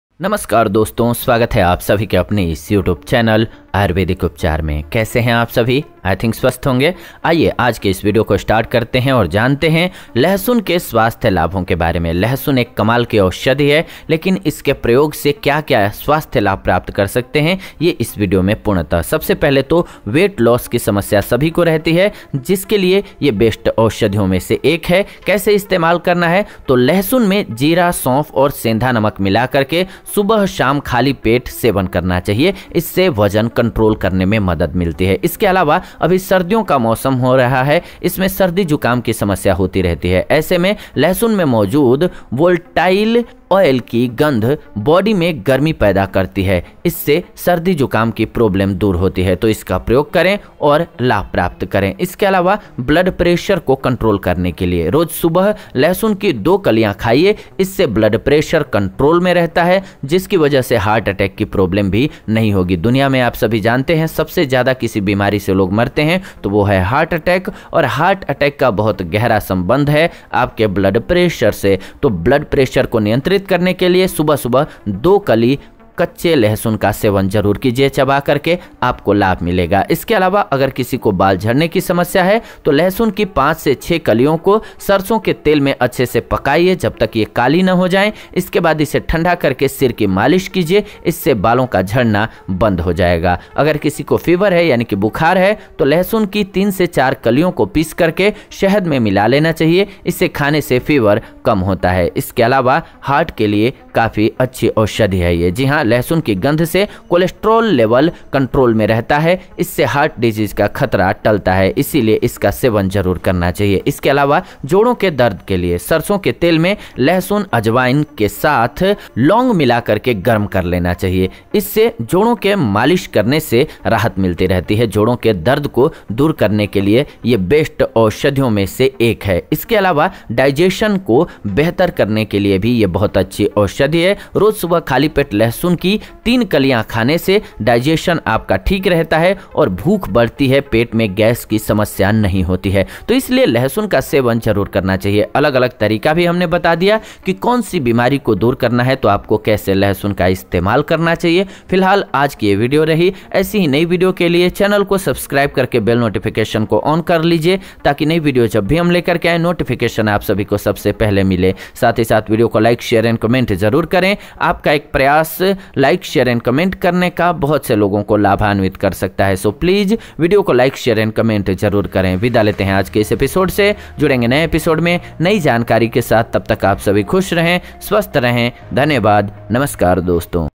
सर्दी-जुकाम, ब्लड प्रेशर, हार्टअटैक, बाल झड़ना, बुखार संबंधी विकार दूर करेगा लहसुन AURVEDIC UPCHAR
Aayurvedik upchar#

गर्भाशय, बच्चेदानी में गांठ या रसौली होने का कारण, लक्षण, इलाज, उपचार, दवा, Fibroid ka ilaj in hindi